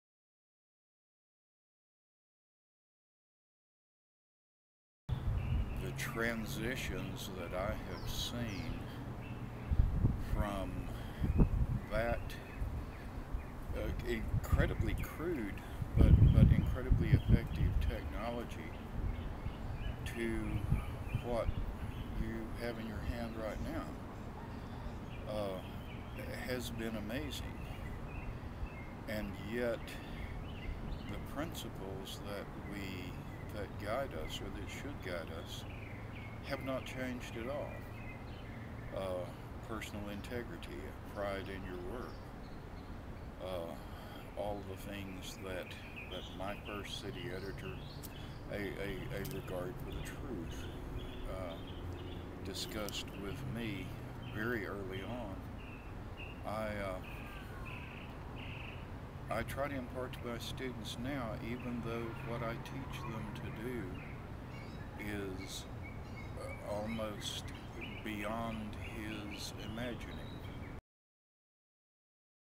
NAHJ Training
Student journalists and professionals gathered at Texas State University to learn Final Cut Express, Video Storytelling and WordPress. Get ready, the industry's changing!

-Meagan O'Toole-Pitts